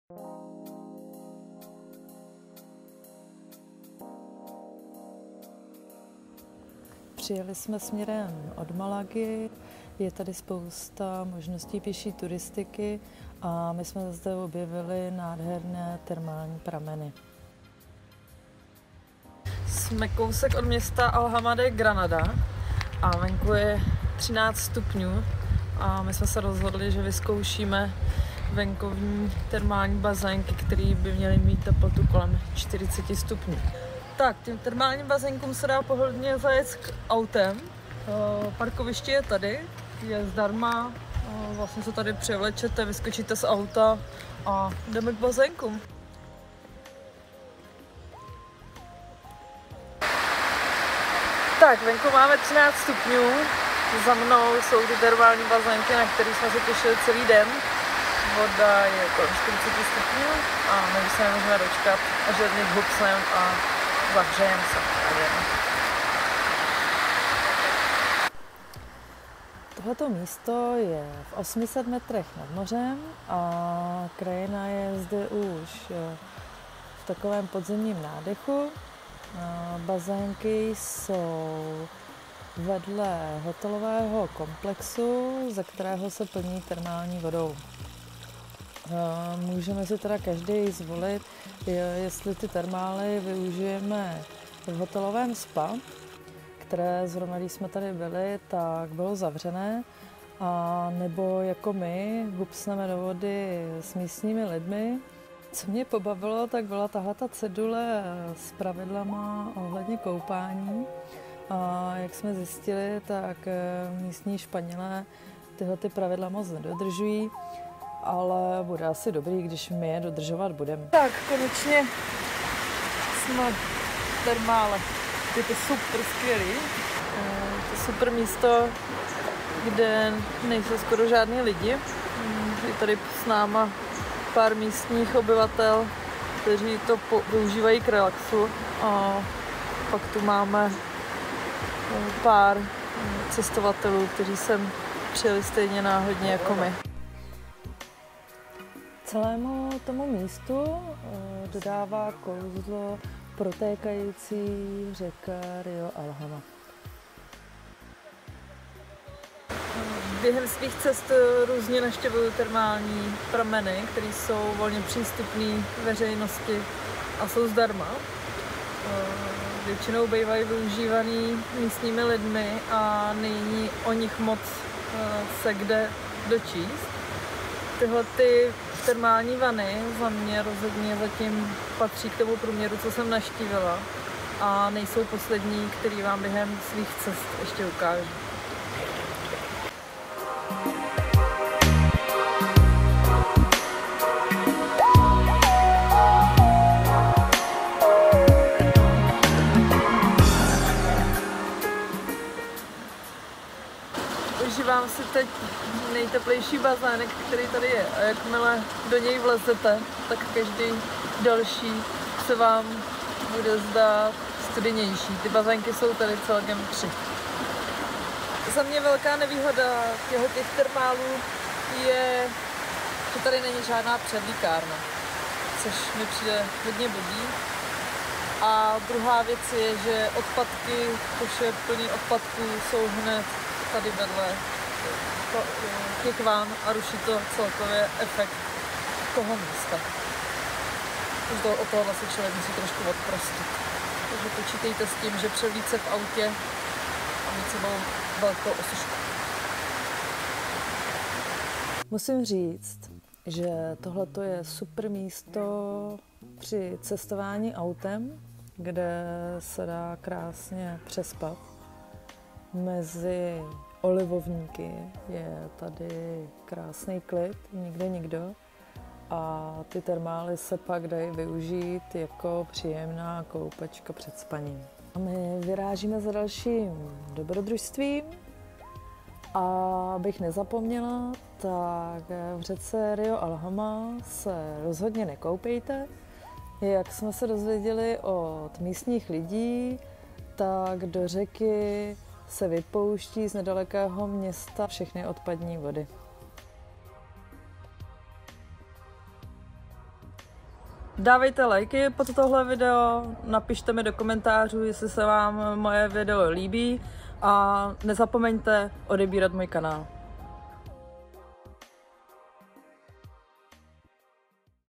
Opuštěné termály v Andalusii - Španělsko
Andalusie a oblast Malagy je plná termálních pramenů a jen některé jsou volně přístupné veřejnosti. My jsme našli jedny z těch, které můžete navštívit. Termály v těchto místech vybudovali Římané, kteří je využívali v doby kdy tady pobývali.
Teplota vody je od 35-40 stupňů.
Najdete je v provencii Granada a dostanete se sem jak od Malagy tak i od Granady. Tohle místo je ideální i na přespání v autě.